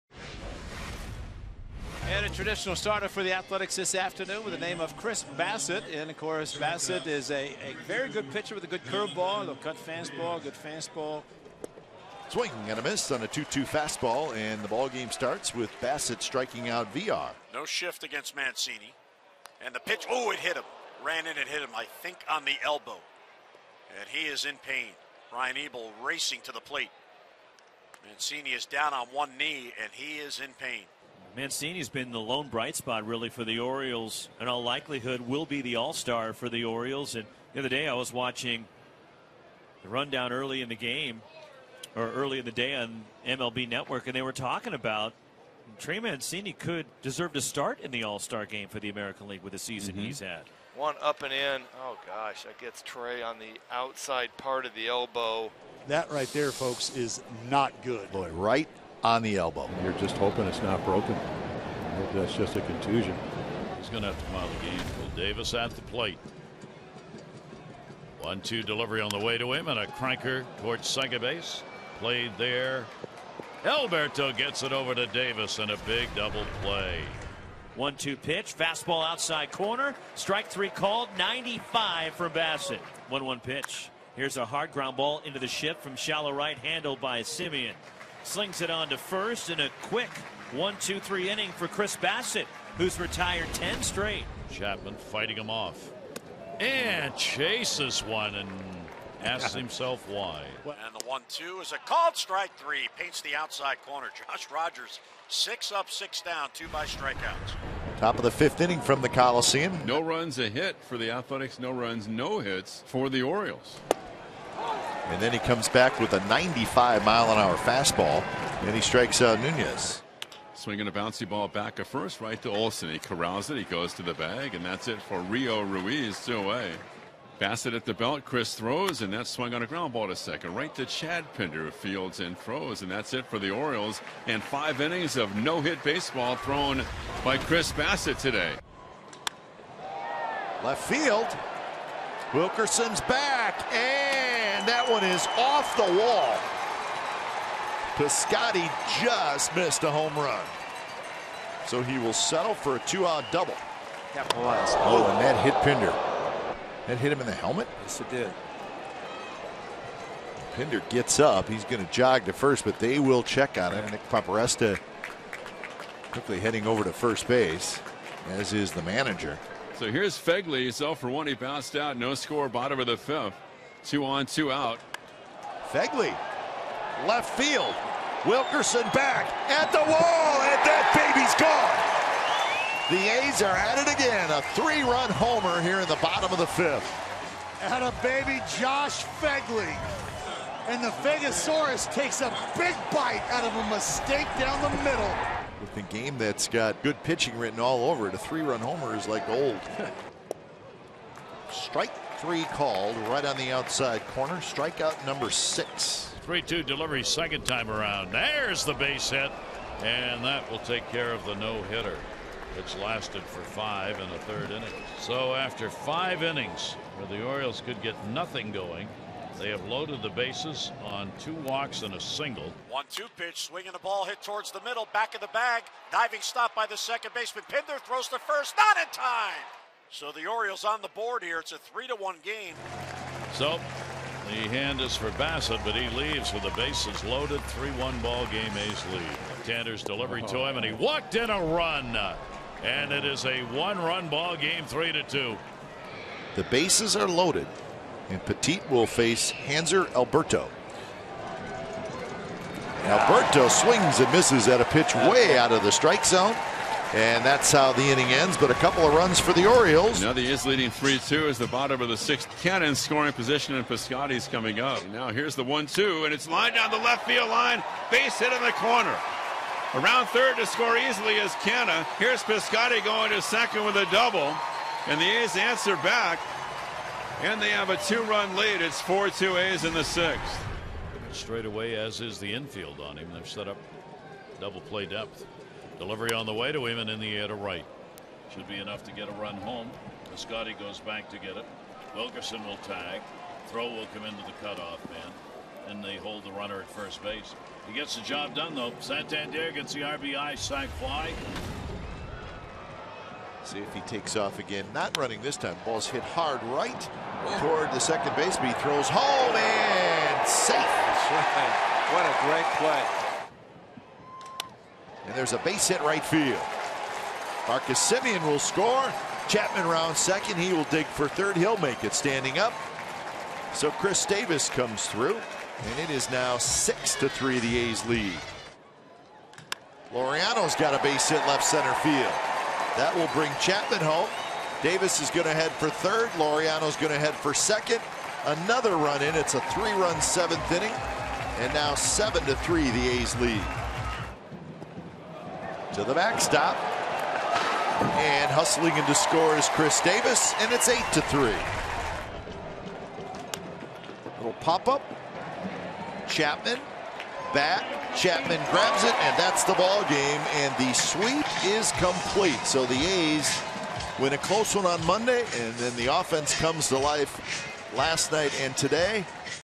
Phegley's HR, Bassitt lead A's | Orioles-A's Game Highlights 6/19/19
Daily Recap: Josh Phegley smashed a three-run homer, while Chris Bassitt tossed 5 2/3 solid frames